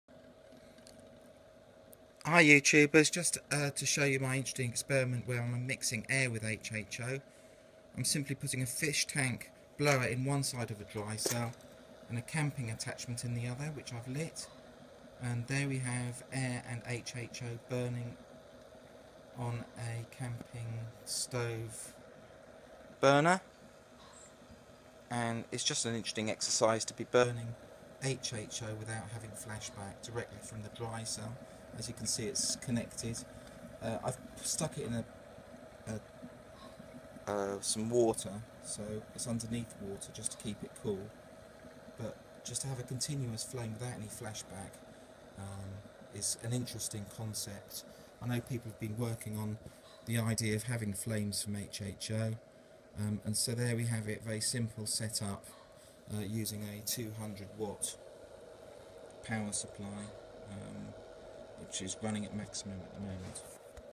HHO Cooking Stove Air Mix Stage 01
A simple set up showing HHO burning with air added from a fish tank blower without flashback. Interesting to see. Campingaz burner. Directly attached to the dry cell sitting underwater to prevent overheating. It is in the black bucket to prevent the breeze from blowing the flame out.